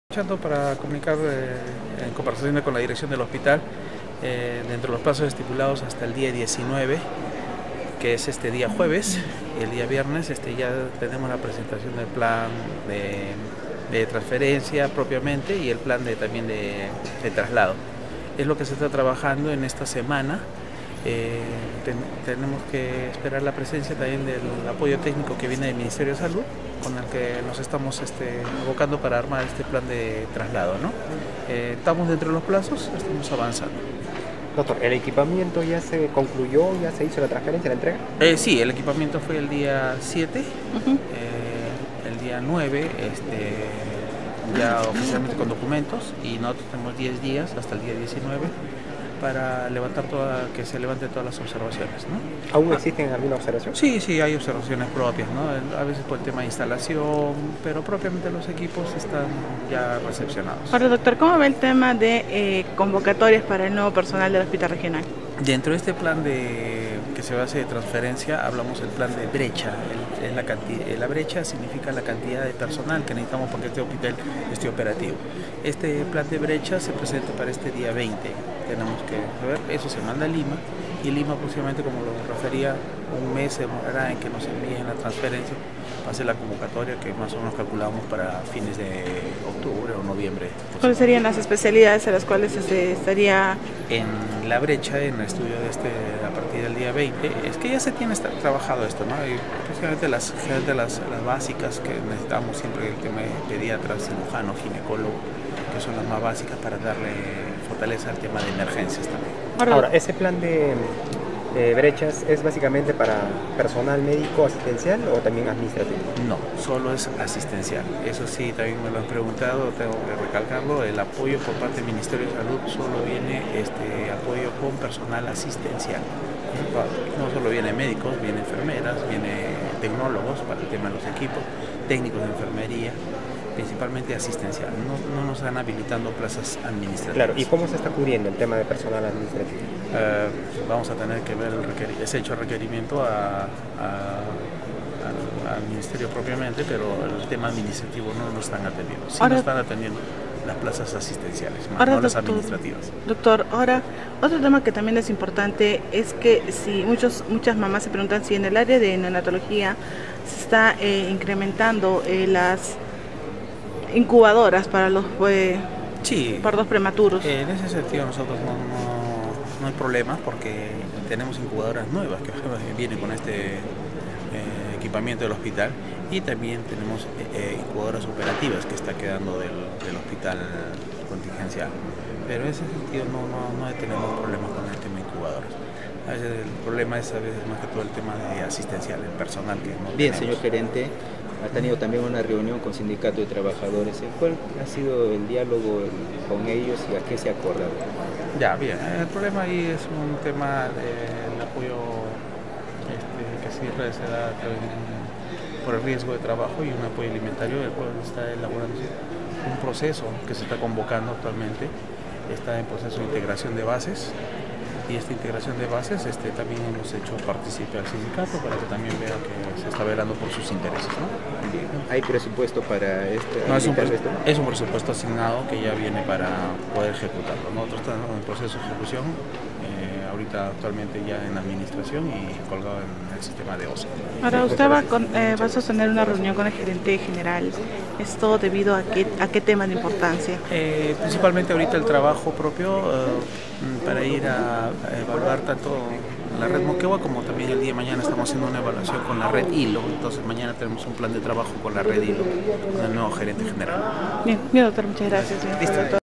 HOSPITAL DE CONTINGENCIA INICIA TRASLADO A NUEVA INFRAESTRUCTURA. 16/09/19
El gerente regional de salud Oscar Galdos Rodríguez señaló que el traslado de los equipos hacia el nuevo hospital regional se viene dando desde el día 7 de septiembre. Así mismo manifestó que aún hay algunas observaciones que levantar revisten riesgo para el funcionamiento del nuevo nosocomio, principalmente en el área biomédico. La semana pasada se concluyó con la etapa de recepción y transferencia del equipamiento médico de forma oficial, explicó Óscar Galdós tras indicar que durante octubre debe cumplirse con este procedimiento Finalmente se refirió al Concurso CAS en el cual se está requiriendo nuevo personal.